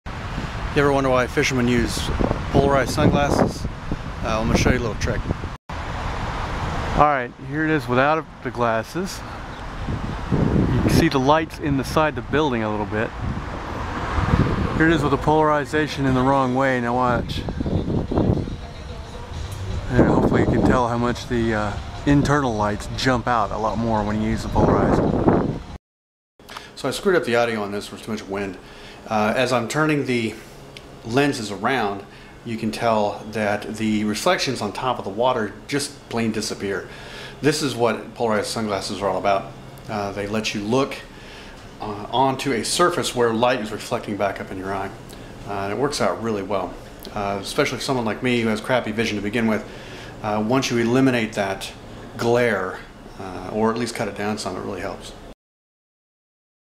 Why do fishermen use polarized sunglasses?
Polarized sunglasses cut the glare